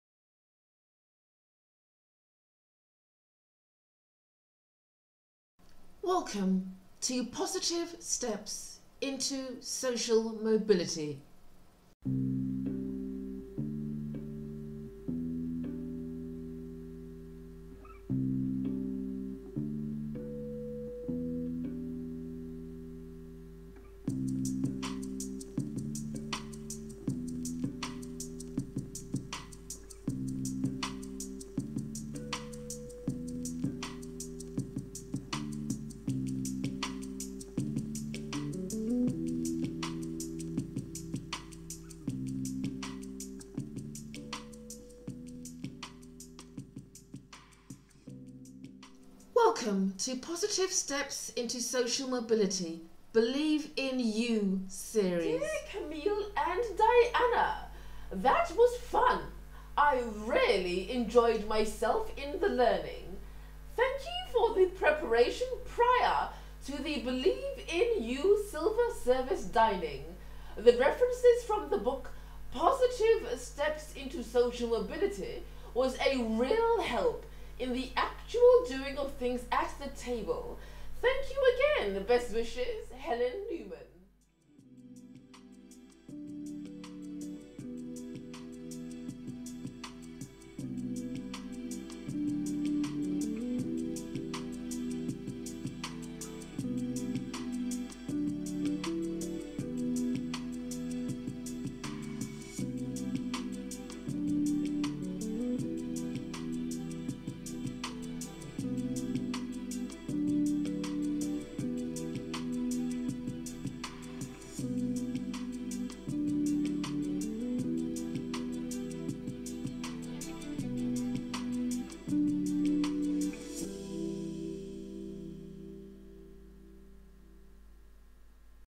Believe In You, May social mobility event
UK Commonwealth Social Mobility.

We have listened to our previous guests and created a "Believe In You" event for 2019.
Did you know that we prefer to do events on a small scale, in comfortable surroundings and welcoming, in order to give our guests the best experience in social mobility steps? Give "Believe In You" a try to help you gain access through social mobility. It is easy.

Experience what our previous guests have enjoyed.
Accepting 11 individuals only per month, so that you are not greatly overwhelmed from the "Believe In You" experience.
Now is the next chance to reserve your place for the next "Believe In You" dining and conversation event.
YOUR positive steps towards a social mobility opportunity is closer than you realise. Especially when you "Believe In You".

I want you to try a successful dining & conversation experience at a "Believe In You" event with Positive Steps Into Social Mobility: Awareness and Empowerment at my risk. Because I know it works but you don’t …yet!
Helping you  get what you want out of life.
Camille Lansiquot and Diana Thulare